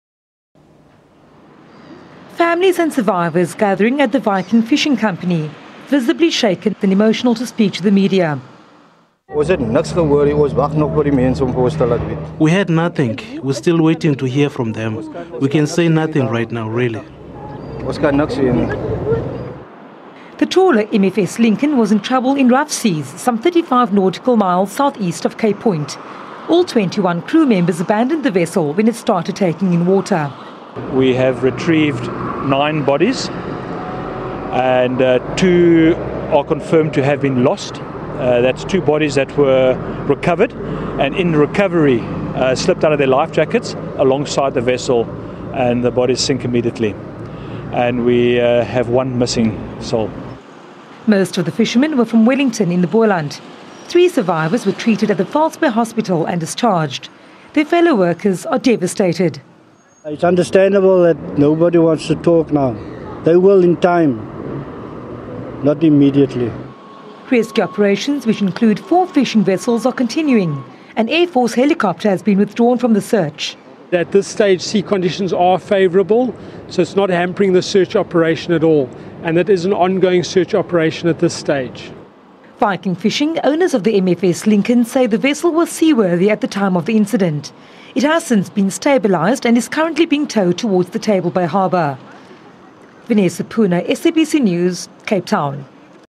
Fishing trawler incident leaves families devastated
The owners of the fishing trawler which ran into difficulty
near Hangklip on the False Bay Coastline on Sunday night, say the incident has left them devastated. Nine crew members drowned and three are missing.